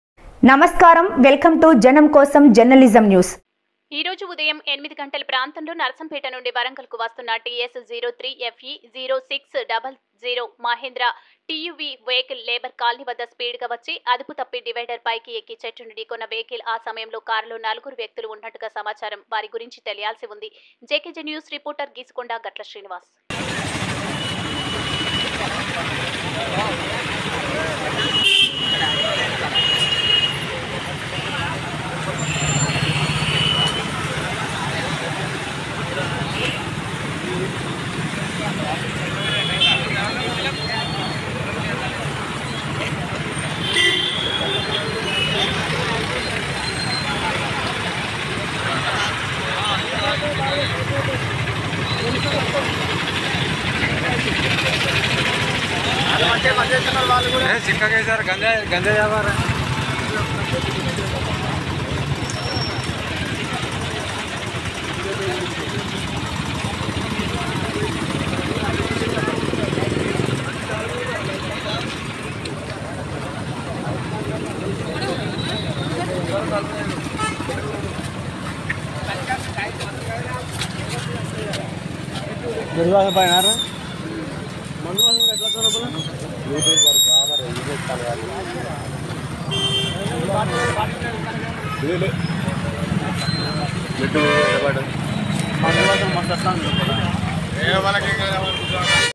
ఈరోజు ఉదయం ఎనిమిది గంటల ప్రాంతంలో నర్సంపేట నుండి వరంగల్ కు వస్తున్న TS 03 FE 0600 మహేంద్ర టియువి....
ఈరోజు ఉదయం ఎనిమిది గంటల ప్రాంతంలో నర్సంపేట నుండి వరంగల్ కు వస్తున్న TS 03 FE 0600 మహేంద్ర టియువి వెహికల్ లేబర్ కాలనీ వద్ద స్పీడ్ గా వచ్చి  అదుపుతప్పి డివైడర్ పైకి ఎక్కి చెట్టును ఢీకొన్న వెహికల్ ఆ సమయంలో కారులో నలుగురు వ్యక్తులు ఉన్నట్టుగా సమాచారం వారి గురించి తెలియాల్సి ఉంది JKJ న్యూస్   రిపోర్టర్ గీసుగొండ గట్ల శ్రీనివాస్